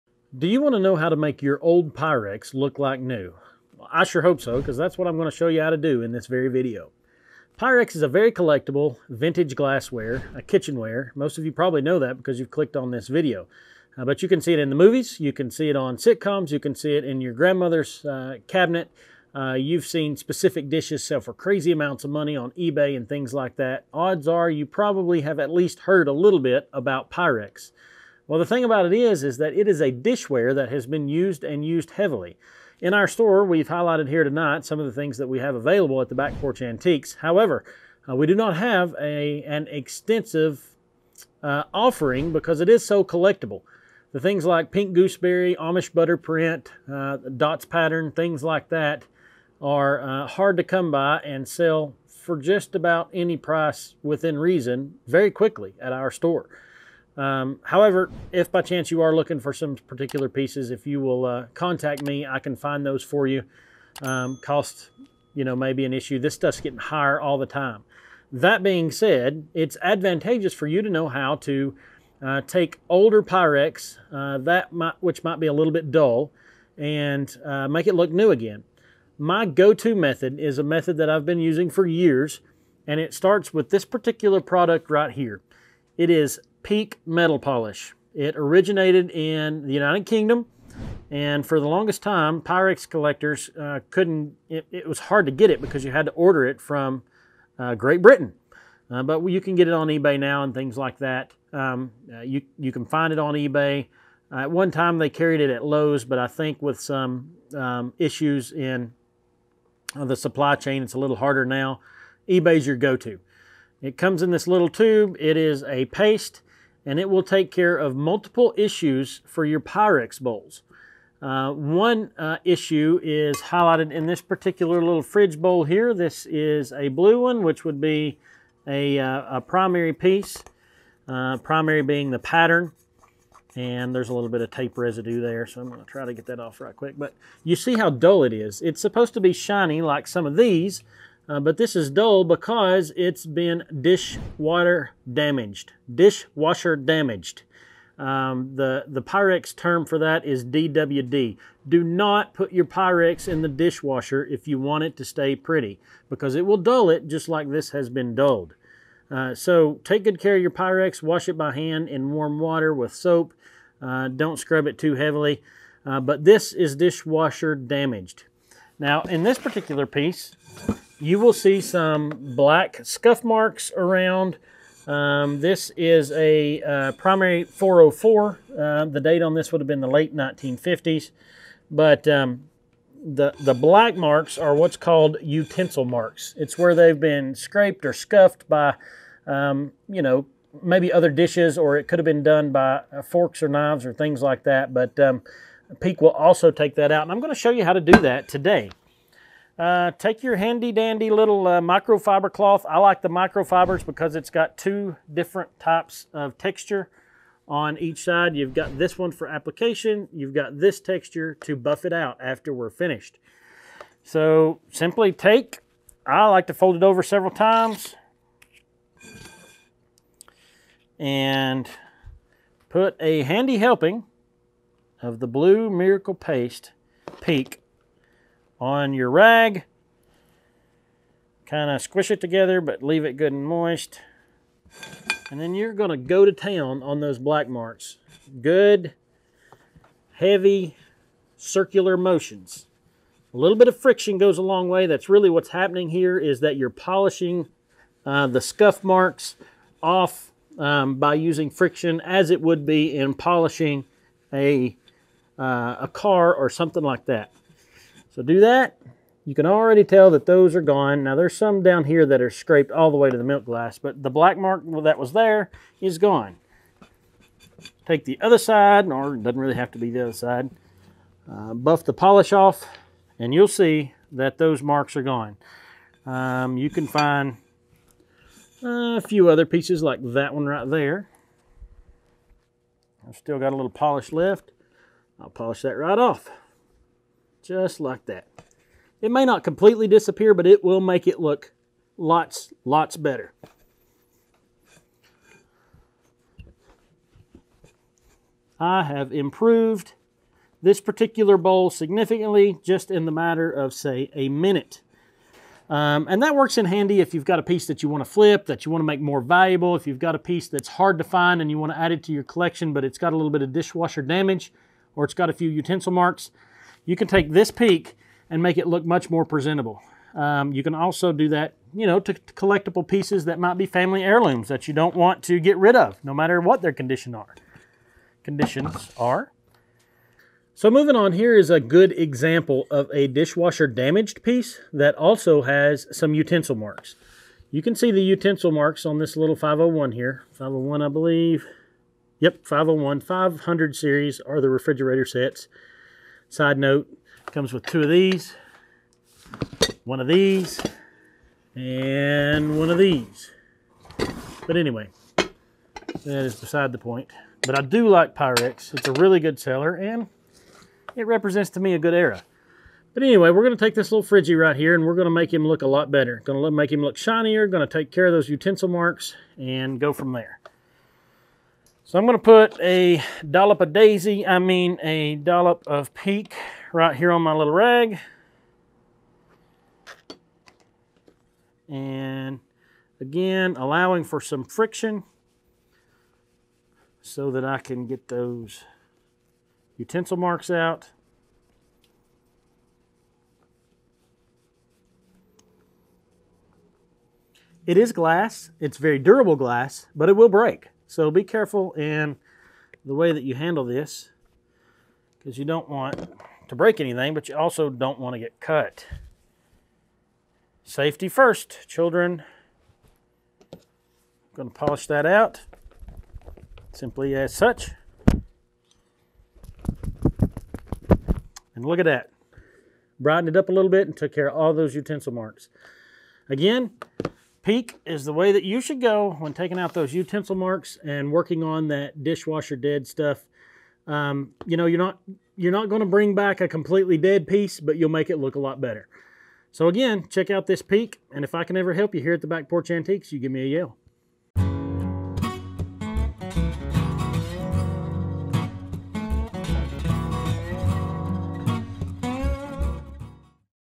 How to Clean Vintage Pyrex | Make Your Vintage Glassware New Again
Have you ever wanted to make your older, used Pyrex look new again? This video show you how to use Peek Metal Polish to clean off some of those wear marks removed and get that Pyrex back to shining like new. Here at the Back Porch Antiques, we want you help you restore your Pyrex to its new condition.

Follow the easy steps we give you in this video to get your vintage glassware shining again. If you want to find the products we use in the video, use the links below to find the towels and Peek metal polish that we use.

Microfiber Cloths
Peek Metal Polish

0:00 - Intro
1:19 - What we use
2:01 - Dish water damaged
3:15 - How to polish
7:07 - Closing thoughts